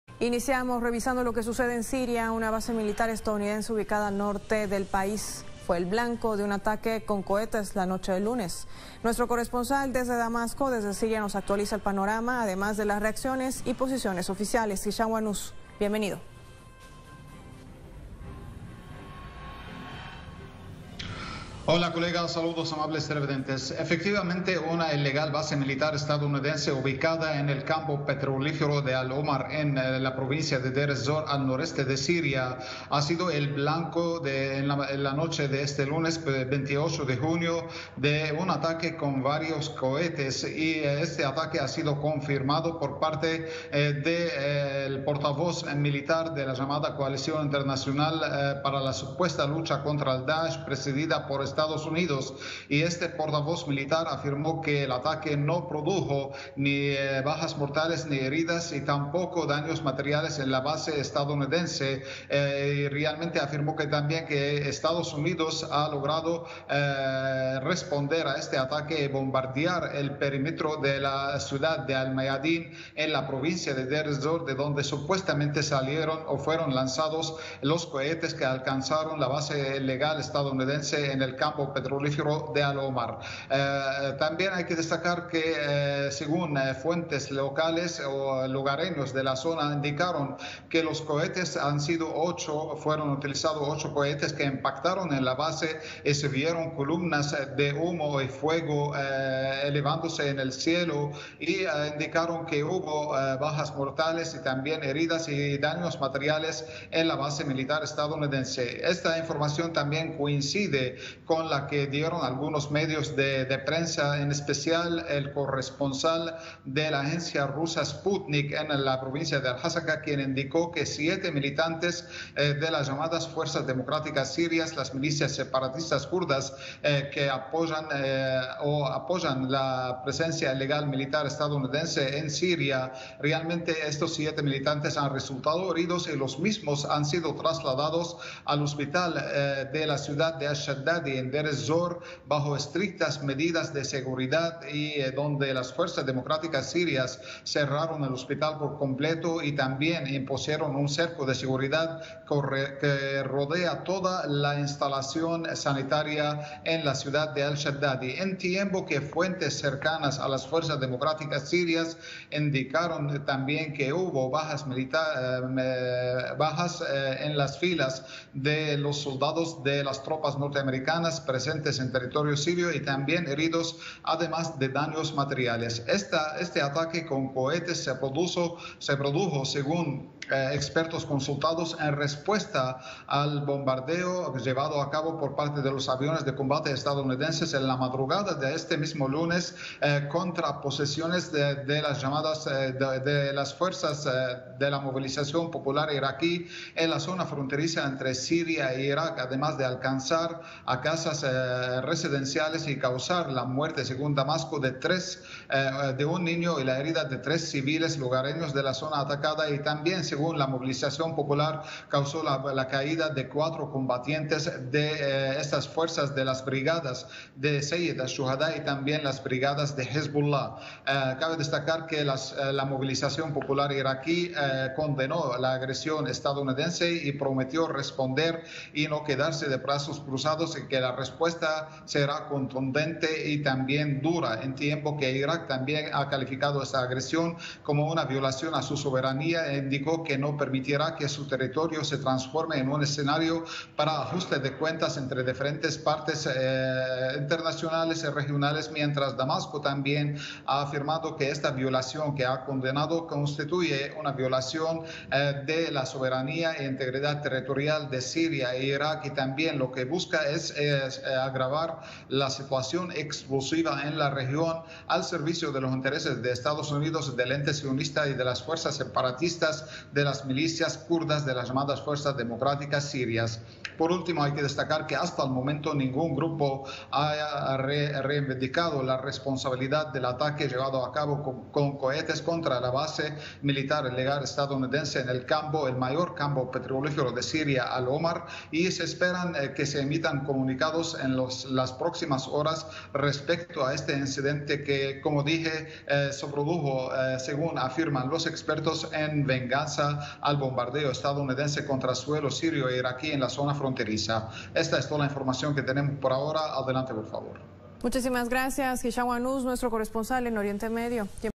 Telesur: attack on US occupation base in Syria (in Spanish)
Atacan base de las fuerzas de ocupación estadounidenses en Siria.
Telesur, 29 June 2021